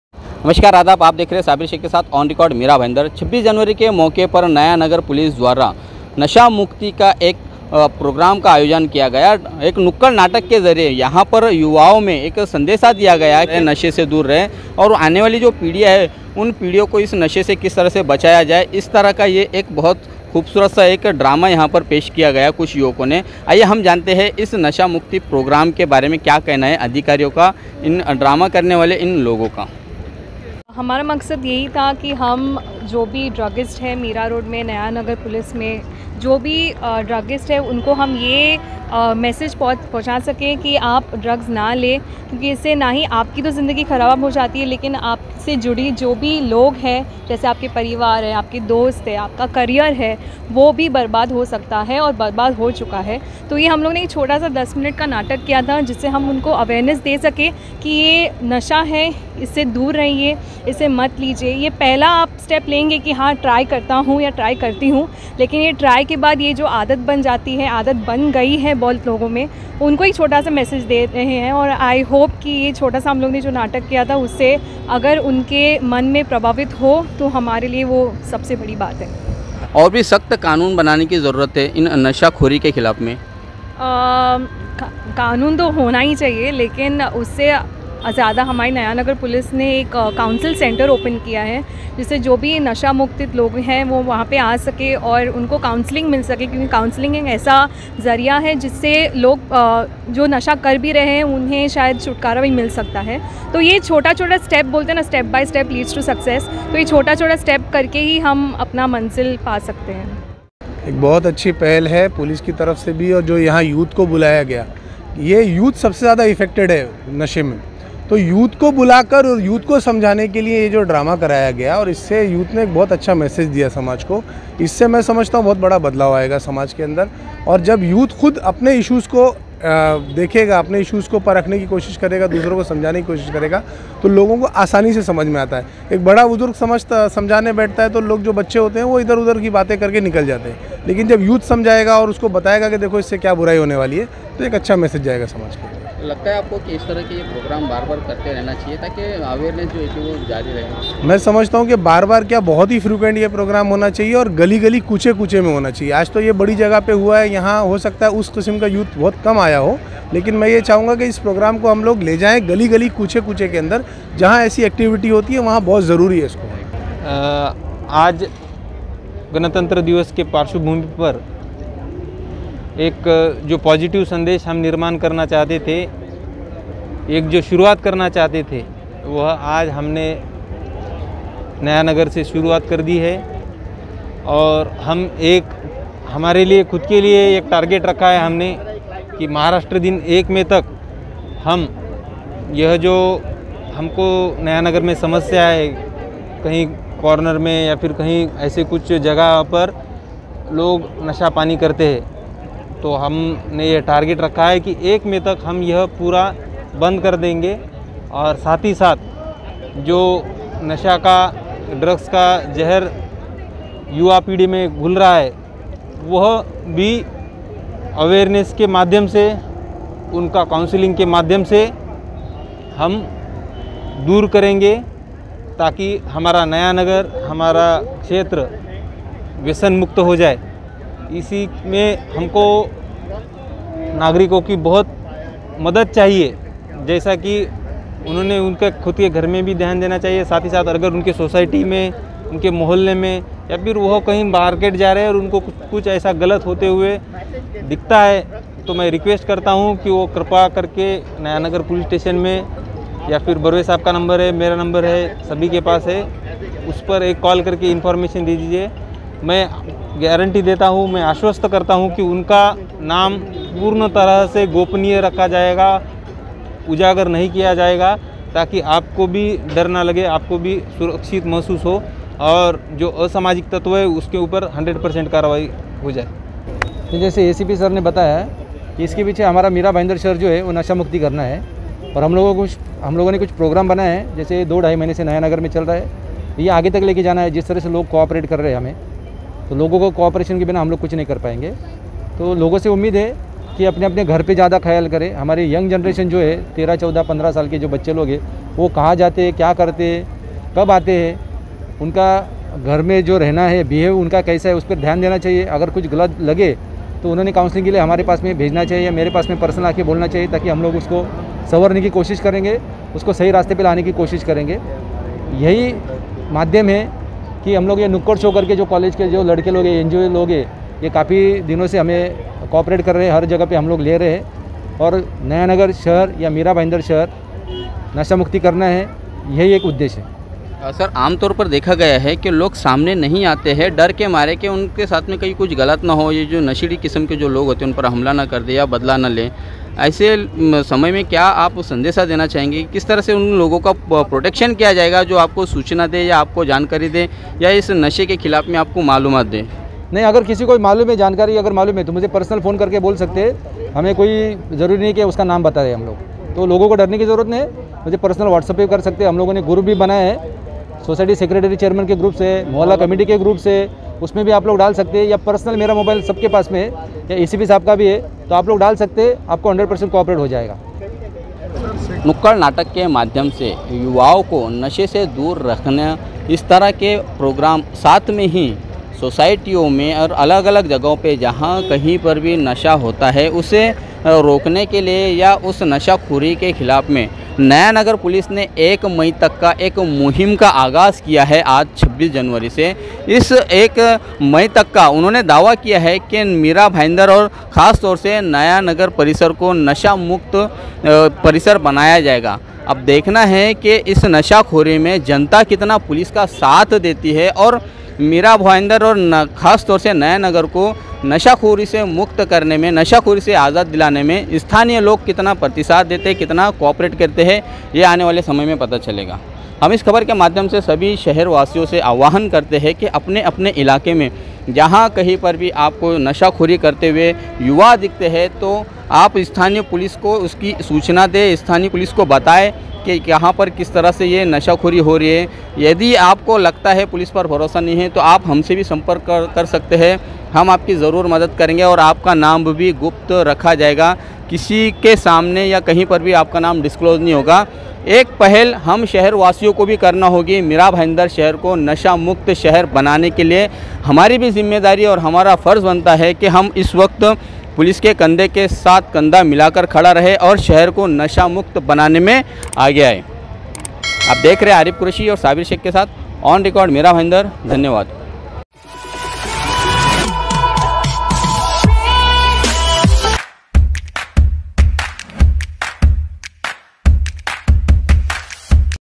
1मई तक नशामुक्त शहर बनाने का पुलिस ने लिया संकल्प | MBVV | MBVV Commissionrate | RepublicDay2021
1मई तक नशामुक्त शहर बनाने का पुलिस ने लिया संकल्प
नयानगर पुलिस ने नशामुक्त इलाका बनाने में उठाए कदम
ब्लेस फाउंडेशन के नुक्कड़ नाटक से युवाओं में जागरूकता
नशेड़ियों की सूचना देने वालो की रखी जाएगी जानकारी गुप्त